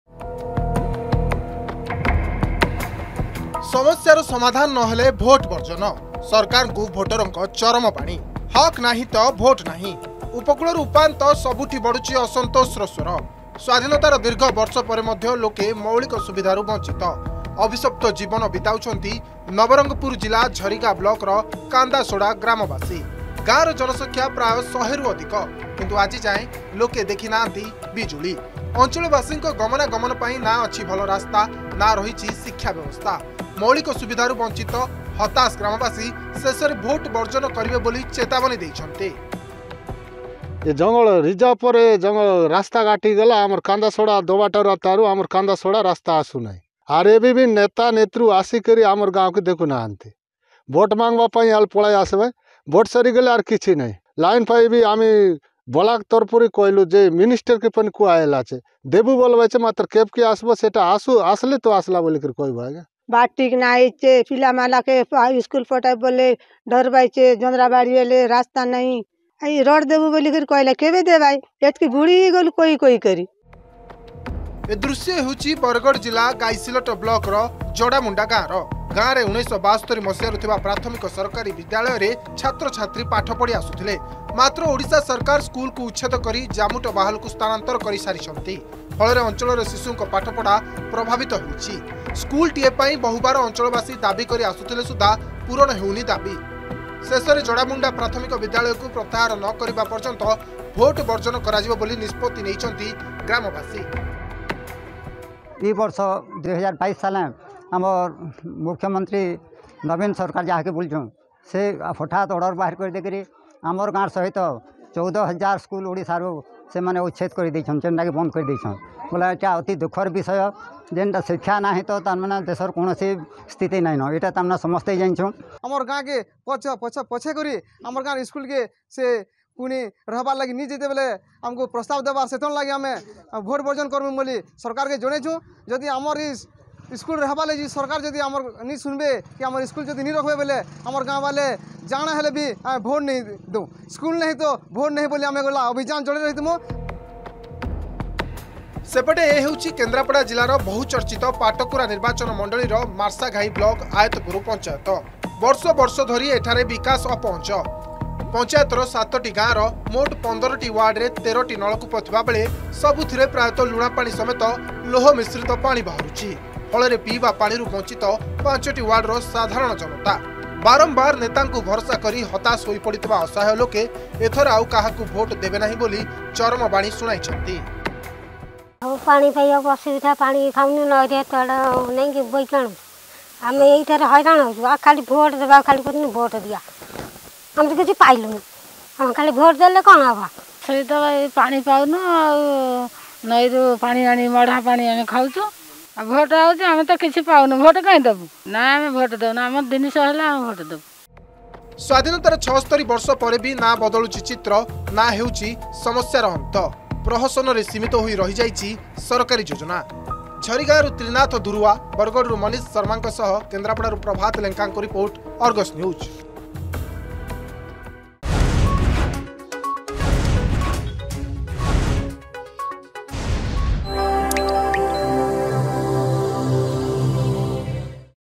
Kendrapada | If Issue Is Not Resolved Then People Will Boycott Election
ସମସ୍ୟାର ସମାଧାନ ନହେଲେ ଭୋଟ ବର୍ଜନ
ସରକାରଙ୍କୁ ଭୋଟରଙ୍କ ଚରମବାଣୀ
ସ୍ବାଧୀନତାର ଦୀର୍ଘ ବର୍ଷ ପରେ ମଧ୍ୟ ଲୋକେ ମୌଳିକ ସୁବିଧାରୁ ବଞ୍ଚିତ । ଅଭିଶପ୍ତ ଜୀବନ ବିତାଉଛନ୍ତି ନବରଙ୍ଗପୁର ଜିଲ୍ଲା ଝରିଗାଁ ବ୍ଳକର କାନ୍ଦାସୋଡ଼ା ଗ୍ରାମବାସୀ । ଗାଁର ଜନସଂଖ୍ୟା ପ୍ରାୟ ୧୦୦ ରୁ ଅଧିକ । କିନ୍ତୁ  ଆଜିଯାଏଁ ଲୋକେ ଦେଖି ନାହାନ୍ତି ବିଜୁଳି । ମୌଳିକ ସୁବିଧାରୁ ବ଼ଞ୍ଚିତ ହତାସ ଗ୍ରାମବାସୀ ଶେଷରେ ଭୋଟ ବର୍ଜନ କରିବେ ବୋଲି ଚେତାବନୀ ଦେଇଛନ୍ତି ।